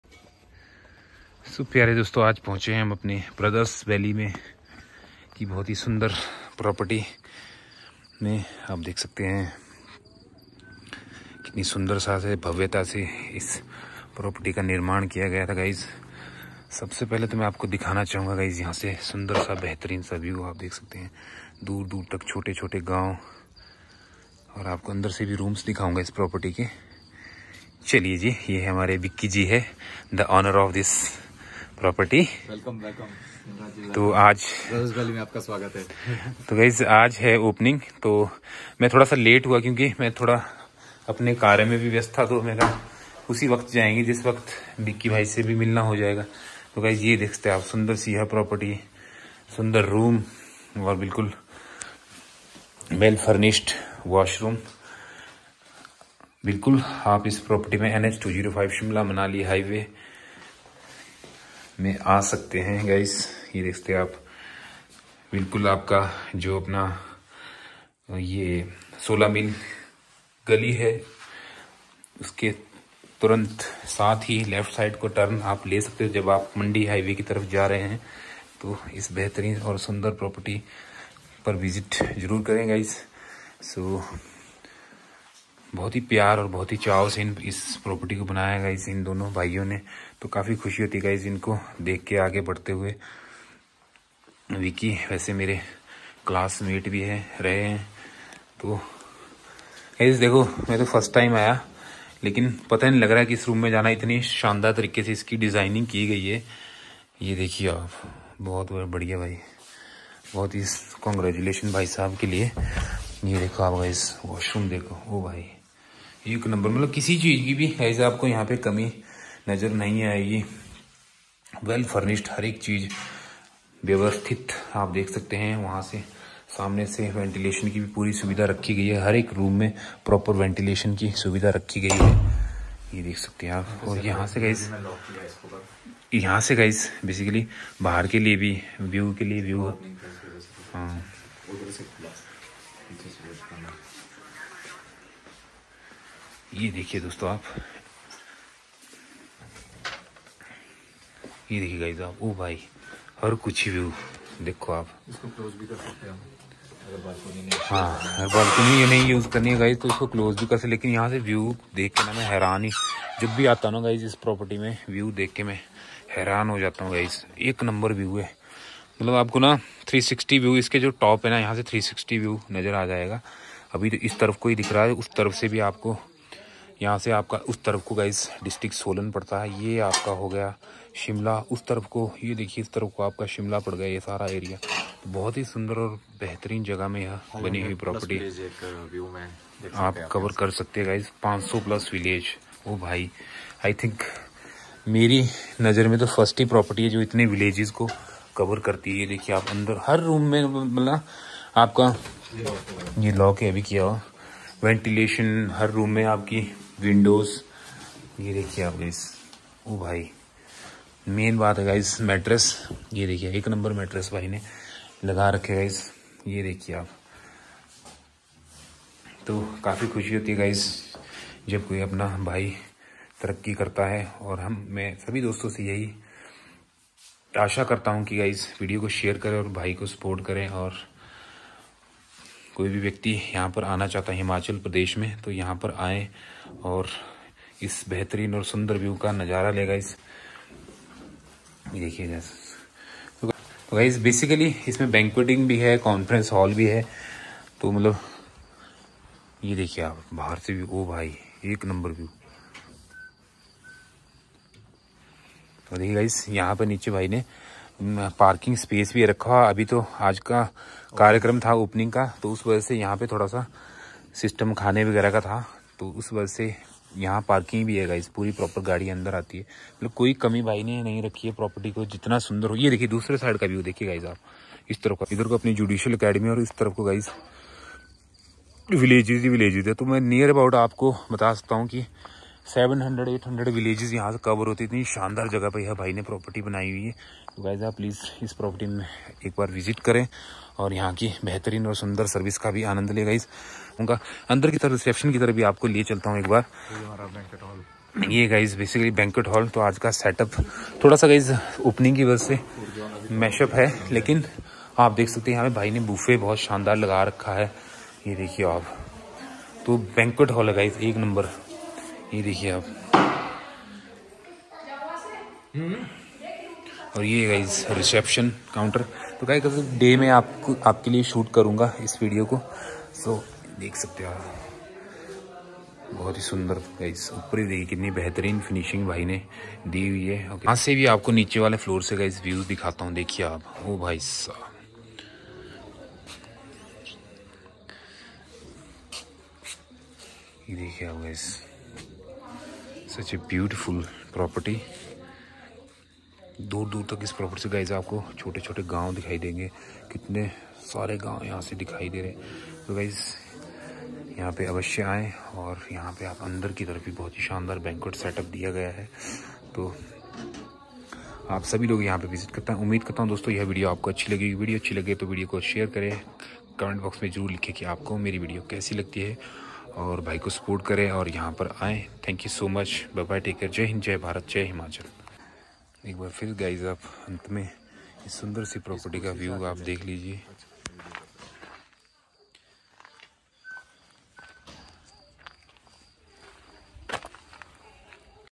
New opening property of my friend . Nh 205 shimla manali highway near solamil gali.
New opening property of my friend.
namaskar Mere Pyare doston doston is video mein aap Dekhenge ki Kis Tarah Se Mere Dost Ne ek Shandar property Banai hai aur agar aap Yahan per Aana Chahte Hain To aap Jarur ine numberon per Sampark Karen yah property Shimla Manali highway Mein judicial Academy Mandal ke bilkul Sath hi sthit hai bahut hi Sundar aur behtarin property Hai Doston aap avashya aaen aur Yahan per visit